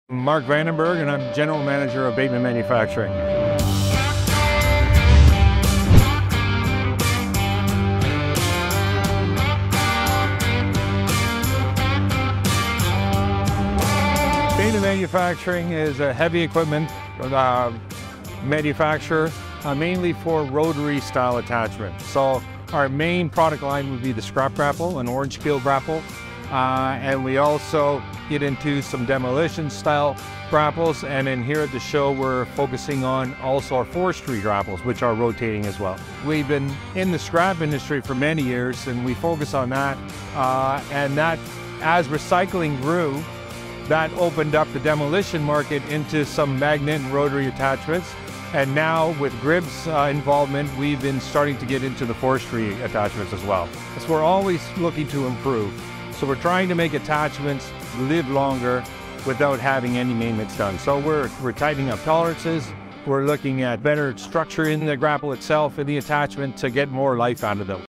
Our Goal? Provide The Best Material Handling Attachments!
Listen to our GM, Mark Vandenberg explain what Bateman Manufacturing aims to do for our customers!